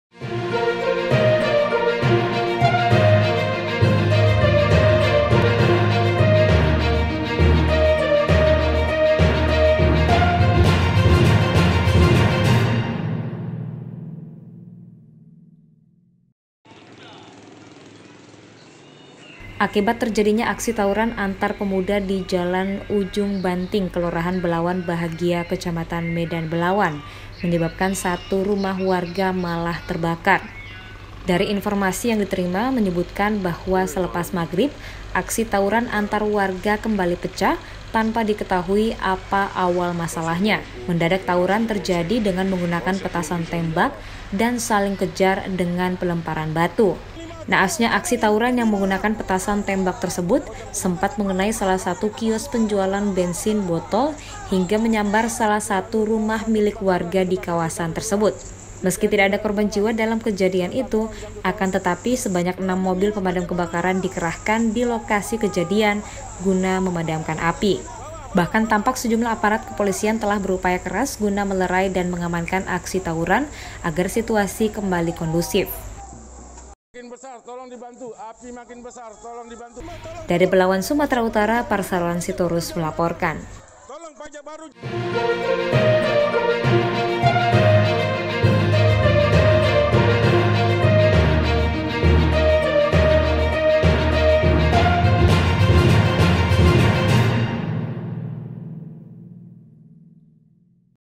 TAWURAN KEMBALI PECAH, RUMAH WARGA TERBAKAR DI BELAWAN
Gegara terjadinya aksi tawuran antar pemuda di jalan Ujung Banting Kelurahan Belawan Bahagia Kecamatan Medan Belawan menyebabkan satu rumah warga malah terbakar / Selasa sore //

Informasi yang diterima menyebutkan,  selepas magrib tadi aksi tawuran antar warga kembali pecah tanpa diketahui apa awal masalahnya //

Mendadak tawuran terjadi dengan menggunakan petasan tembak dan saling kejar dan pelemparan batu //

Naasnya aksi tawuran yang menggunakan petasan tembak tersebut sempat mengenai salah satu kios penjualan bensin botol hingga menyambar salah satu rumah milik warga di kawasan tersebut //

Meski tak ada korban jiwa dalam kejadian itu akan tetapi sebanyak 6 mobil pemadam kebakaran dikerahkan di lokasi kejadian guna memadamkan api //

Bahkan tampak sejumlah aparat kepolisian telah berupaya keras guna melerai dan mengamankan aksi tawuran agar situasi kembali kondusif //

"Saat ini situasi sudah kembali aman dan kondusif setelah diamankan aparat kepolisian serta kobaran api telah berhasil dipadamkan / kata salah seorang warga saat berada di lokasi kejadian tersebut //